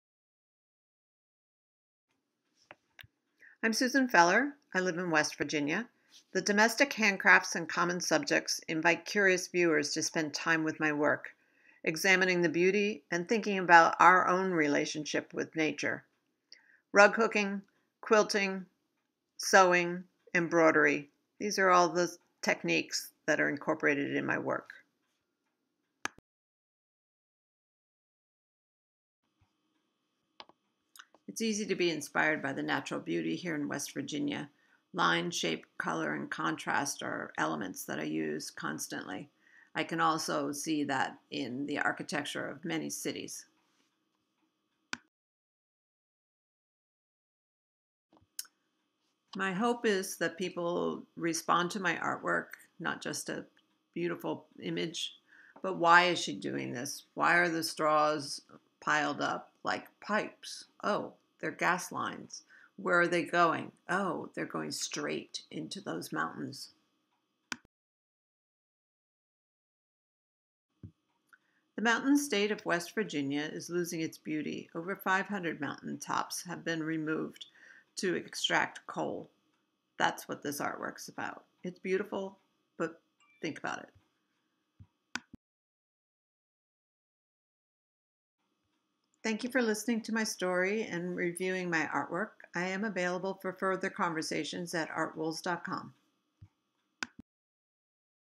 Susan L Feller profile
Introduction to the subjects and style of Susan L Feller's fiber art: environmental and  social issues, the natural beauty of Appalachia, traditional rug hooking and applique' in contemporary combinations